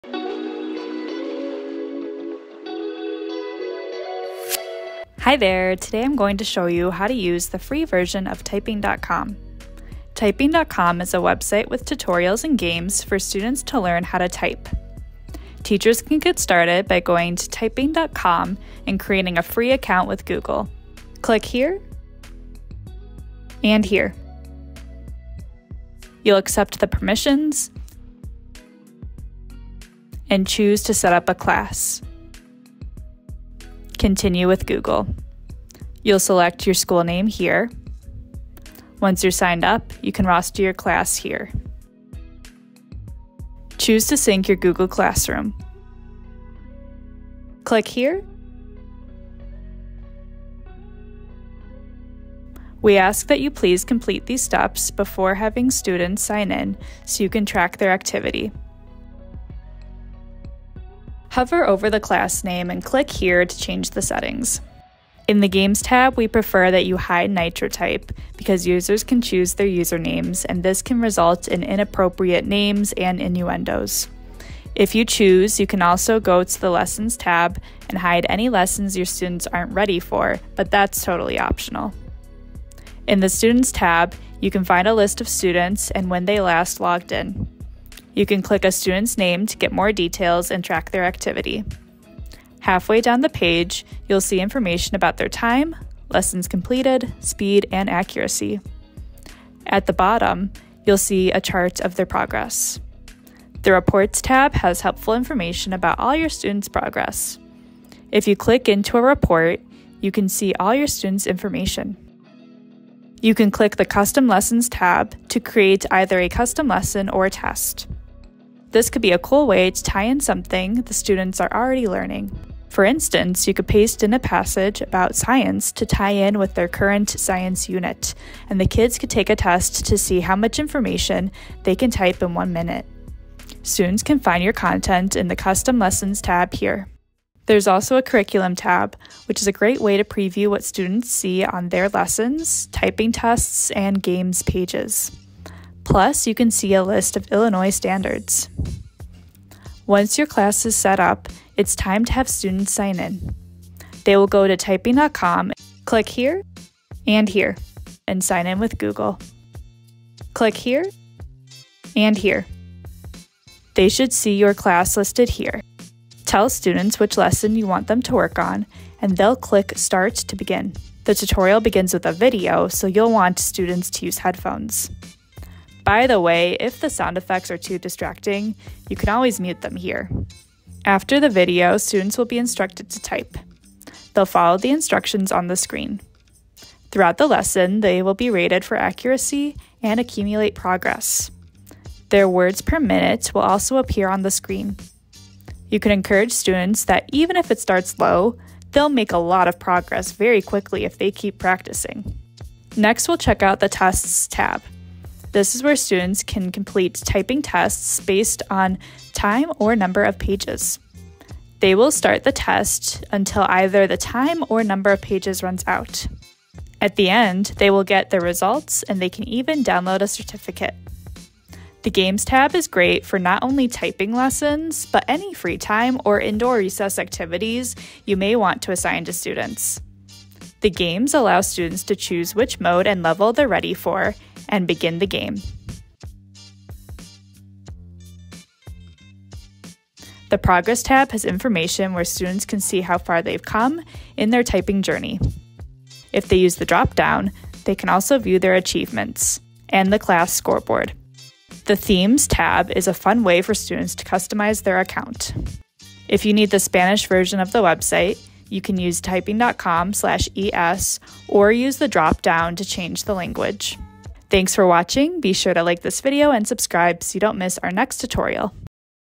How Get Started with Typing.com | Tutorial for Teachers
Here is everything you need to know about getting started with Typing.com!

Timestamps:
00:00 Intro
0:15 Create an account
0:38 Roster your class
1:00 Class settings
1:22 Student information
1:43 Reports
1:52 Custom lessons
2:17 Curriculum
2:29 How students sign in
2:46 Starting a lesson
3:23 Tests
3:41 Games
4:00 Progress
4:11 Themes
4:16 Changing the language to Spanish